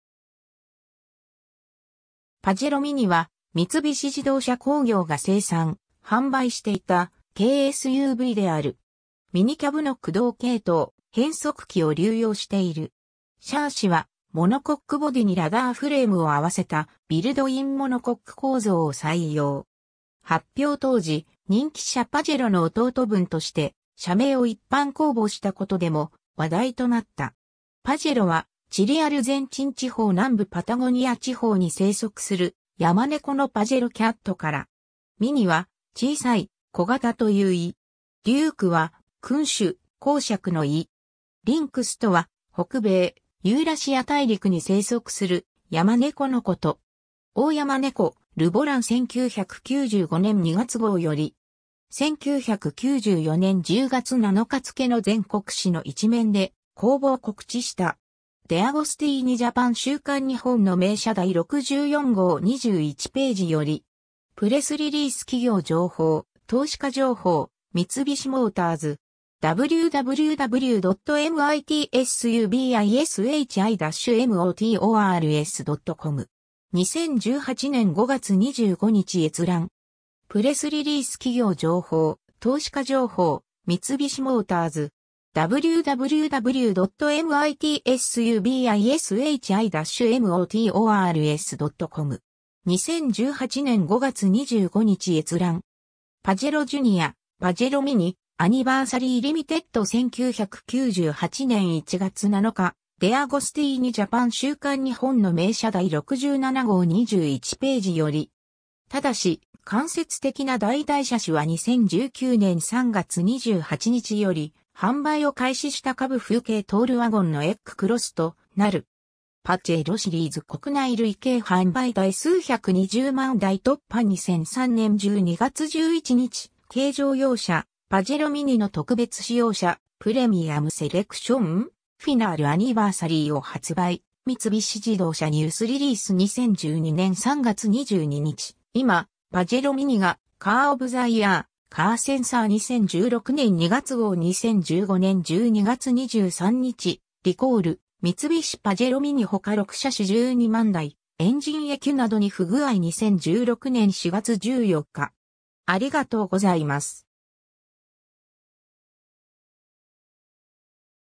三菱・パジェロミニ
パジェロミニ（Pajero Mini）は、三菱自動車工業が生産・販売していた軽SUVである。
 ミニキャブの駆動系統・変速機を流用している。
シャーシはモノコックボディにラダーフレームを合わせたビルドインモノコック構造を採用。
 発表当時、「人気車パジェロの弟分」として、車名を一般公募したことでも話題となった。
 (Sources: Facts & Figures 2000, Facts & Figures 2002, Facts & Figures 2005, Facts & Figures 2009,Facts & Figures 2013,Facts & Figures 2017, Facts & Figures 2018 Mitsubishi Motors website) 「パジェロ」（PAJERO）はチリ・アルゼンチン地方南部パタゴニア地方に生息するヤマネコのパジェロキャット（またはパンパスキャット、コロコロ）から。
 「ミニ」（MINI）は「小さい」、「小型」という意。
 「デューク」（DUKE）は、君主、公爵の意。
 「リンクス」（LYNX）とは、北米、ユーラシア大陸に生息するヤマネコのこと。
→オオヤマネコ ^ ｢ル・ボラン｣1995年2月号より。
1994年10月7日付の全国紙の一面で公募を告知した。
 ^ デアゴスティーニジャパン 週刊日本の名車第64号21ページより。
^ “プレスリリース | 企業情報・投資家情報 | MITSUBISHI MOTORS” (日本語). 2018年5月25日閲覧。
 ^ パジェロジュニア／パジェロミニ「アニバーサリーリミテッド」1998年01月07日 ^ デアゴスティーニジャパン 週刊日本の名車第67号21ページより。
 ^ ただし、間接的な代替車種は2019年3月28日より販売を開始したCUV風軽トールワゴンのeKクロスとなる。
 ^ 「パジェロシリーズ」国内累計販売台数120万台突破 - 2003年12月11日 ^ 軽乗用車 『パジェロミニ』 の特別仕様車「Premium Selection ～Final Anniversary～」を発売 - 三菱自動車 ニュースリリース 2012年3月22日 ^ 今、パジェロミニ がカーオブザイヤー？…カーセンサー2016年2月号2015年12月23日 ^ 【リコール】三菱 パジェロミニ ほか6車種12万台、エンジンECUなどに不具合2016年4月14日